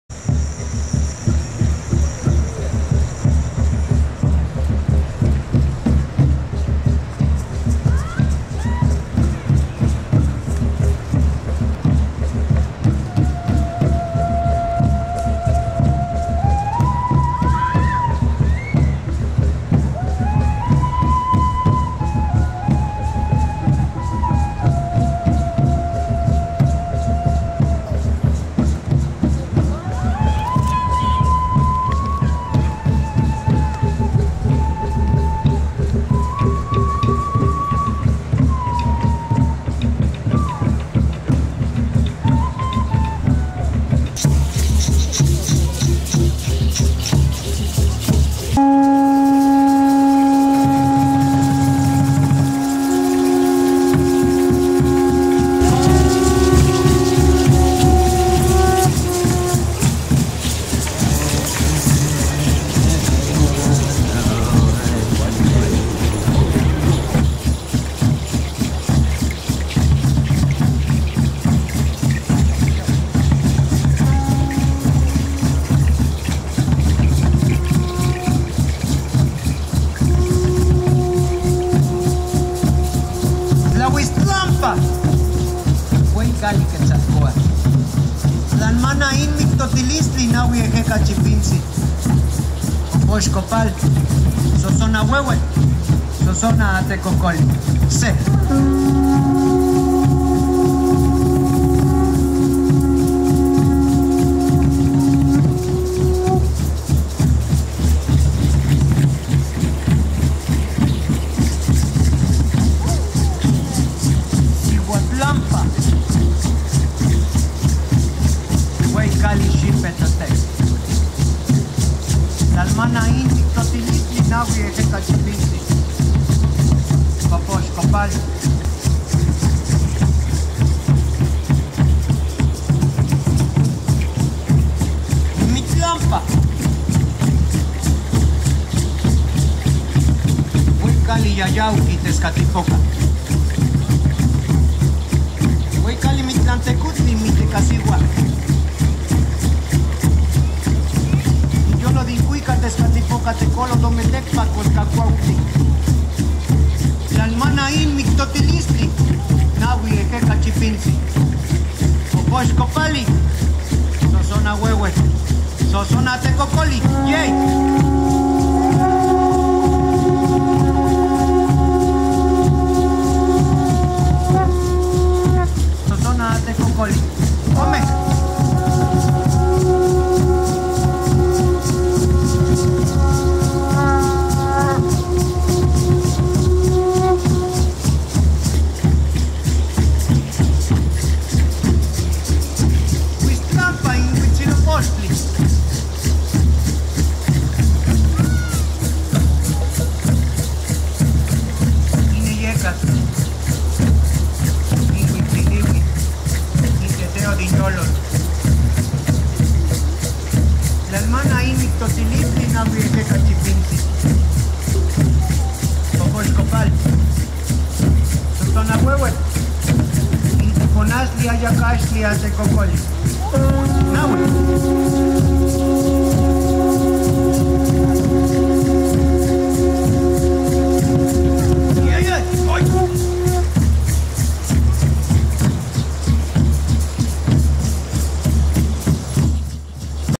Omeyocan Dance title cards included
The Omeyocan Dance Company performed a ritual Earth blessing ceremony in Netarts August of 2022. Here are some highlights from the event. Enjoy!